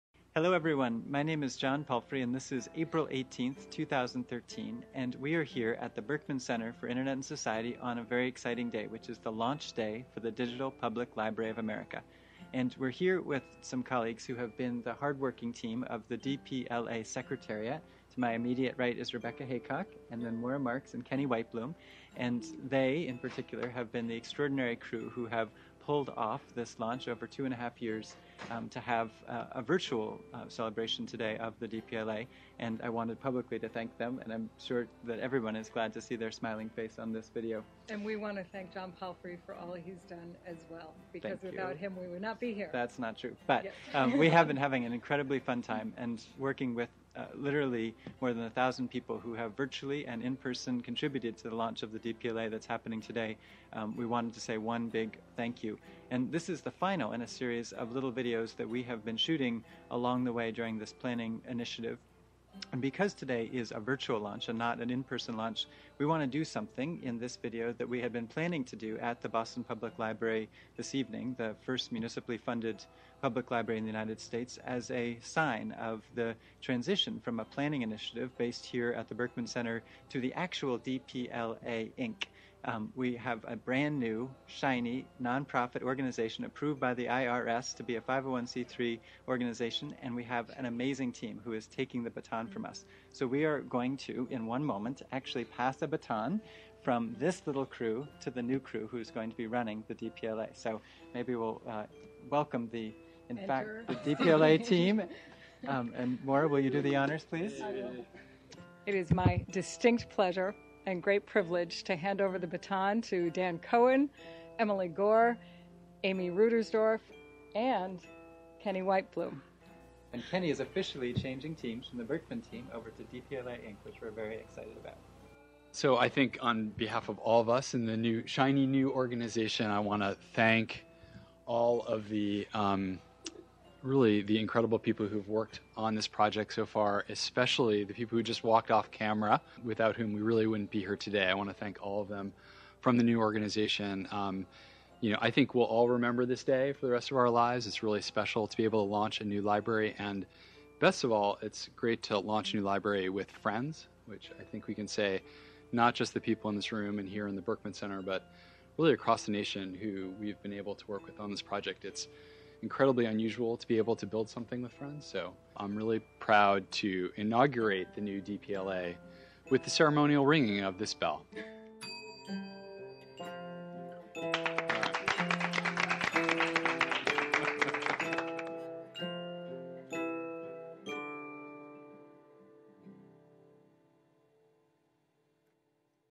April 18, 2013 Update from the Digital Public Library of America
John Palfrey hands off the reins of the Digital Public Library of America from one team to another, as the DPLA is officially launched as a web tool and a new organization, led by Dan Cohen.